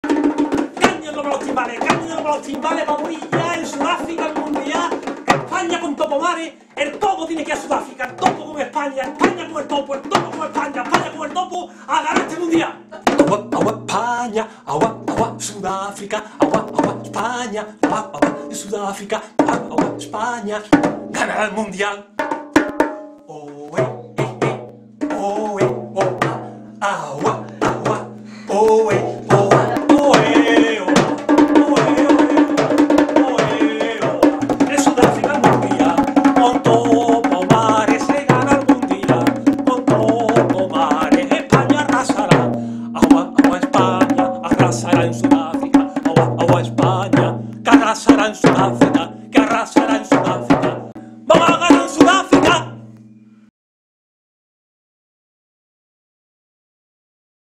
Topomares el de los timbales. MUNDIAL SUDAFRICA 2010
Rumbo a Sudáfrica 2010 dando caña a los timbales .

NO CUESTA NADA!

¡¡¡¡PODEMOS!